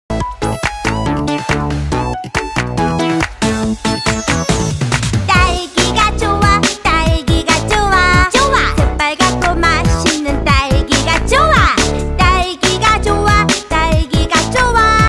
가내수공업 딸기 아이스바
소규모 매장에서 직접 만드는 딸기 아이스바!!!
이거 먹고 나면 다른거 못먹음(뻥) ㅎ